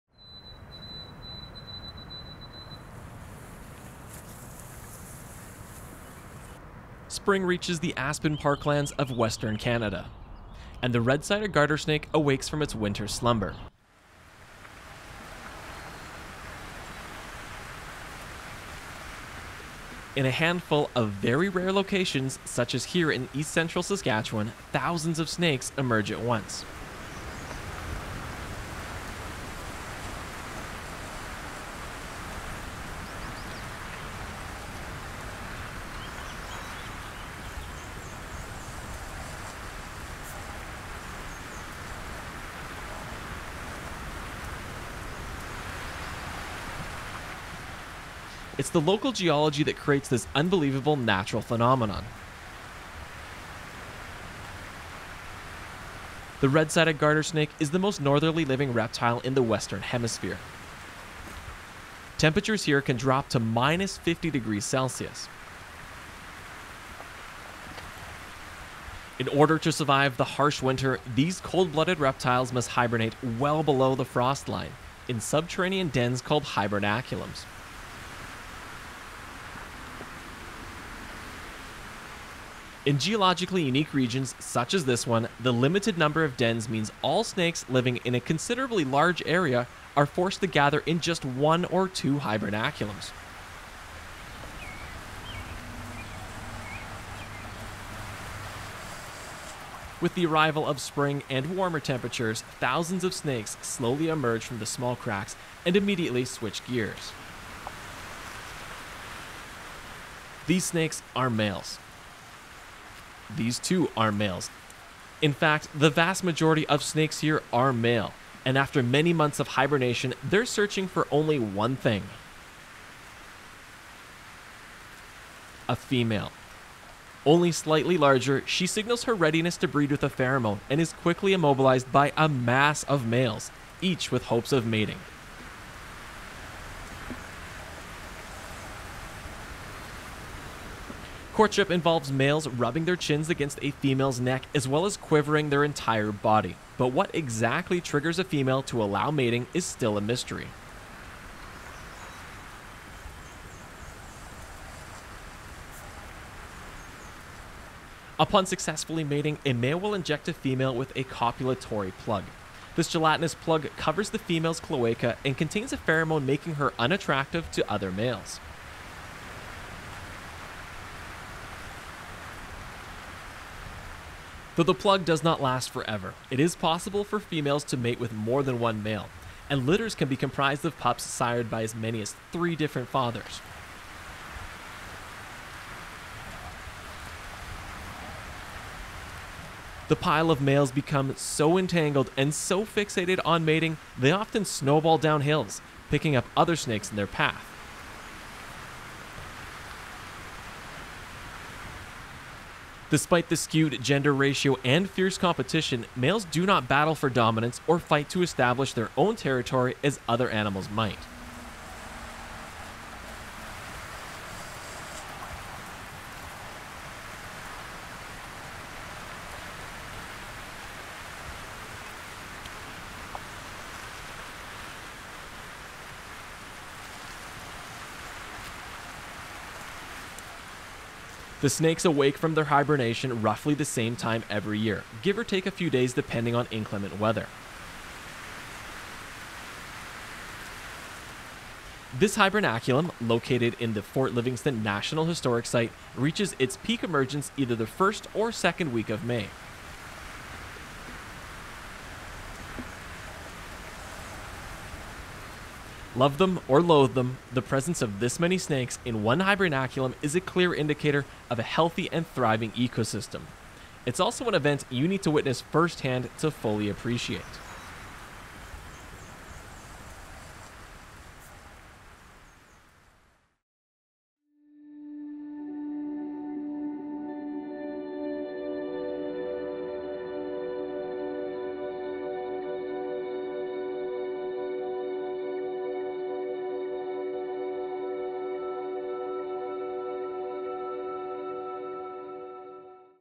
30,000 Snakes of Fort Livingstone
A gathering of this many snakes is not a common occurrence and only takes place in a handful of special sites in North America. The red-sided garter snake is the most northerly living reptile in the western hemisphere and must endure winter temperatures below minus fifty degrees Celsius. In order to survive the harsh cold, the snakes hibernate well below the frost line – in subterranean dens called hibernaculums. In geologically-unique regions, such as here in eastern Saskatchewan and the more well known Narcisse site in Manitoba, there are a very limited number of dens and all the snakes living in the surrounding area are forced to gather in one or two hibernaculums.